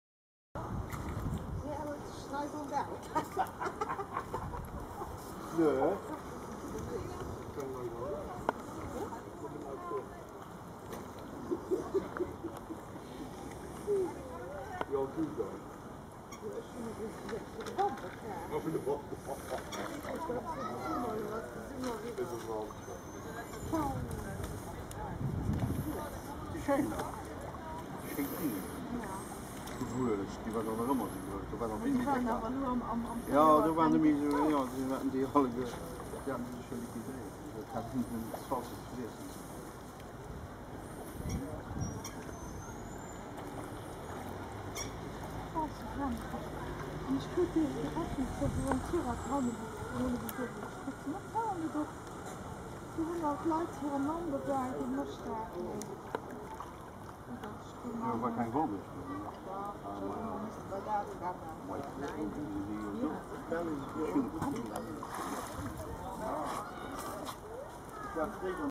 Huge Shoal of Mullet being fed on bread in Majorca Marina
Amazing sight to see these fish feeding in the local marina